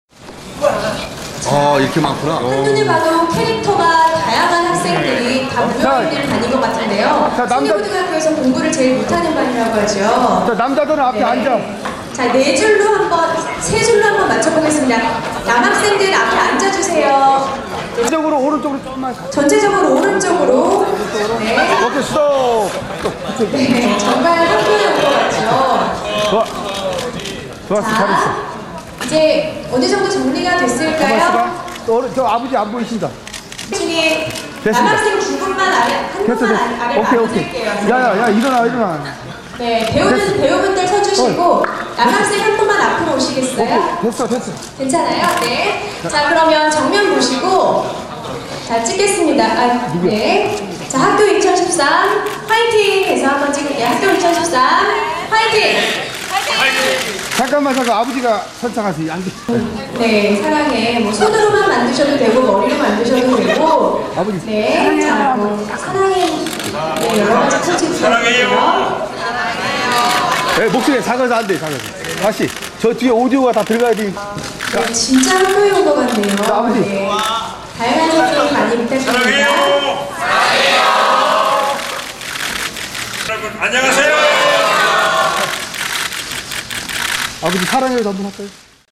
학교2013 2학년 2반 제작발표회 포토타임 ㅣ KBS방송
서울 시내 178개 고등학교중 최하위권인 승리고등학교 문과 꼴찌 2학년2반!
여러분에게 즐거움과 감동을 선사할 '학생떼거리'를 소개합니다!